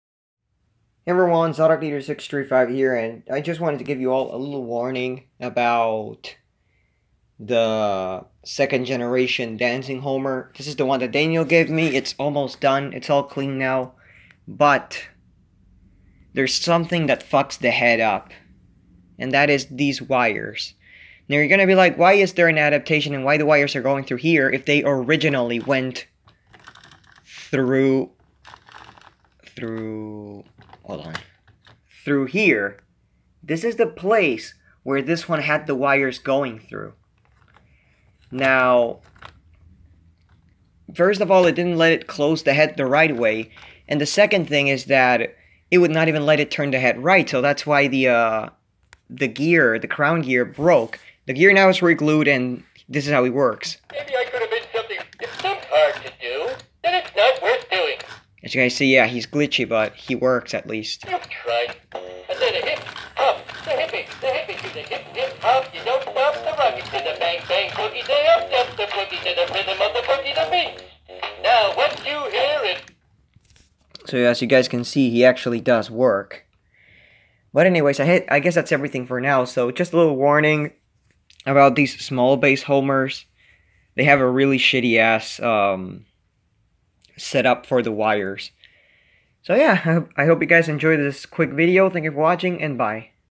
warning about the small base homers
basically wires go in the head through the side, and that ain't clever designing.